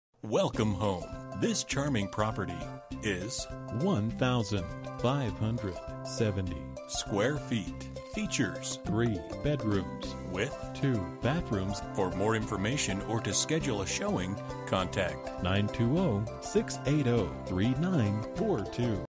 Homes for sale - 316 Butterfield, WRIGHTSTOWN, WI 54180
Another high quality new construction by Vans Construction of Appleton on a quiet cul-de-sac in a Wrightstown subdivision. The home is our Dakota model & features an awesome floor plan w/1570 sqft, 3 bdrms, 2 full bths. The kitchen offers maple cabinets, pantry, tile backsplash & an island with granite countertops. Patio doors off the dinette lead to the good sized backyard. This flows into the spacious great room with gas fireplace. The master suite has a lrg walk-in closet & private bathroom with double sink vanity. 1st floor laundry, 3 car garage. Brick/vinyl exterior. July 2020 complete.

Bedrooms: 3
Bathrooms: 2
Square Feet: 1570
Price: $284,900

For more information about this property, please contact Tom Monahan at or . You can also text 6156609 to 67299.

TomMonahanHomes.com
04/27/2020 02:56:02 pm

Last modified: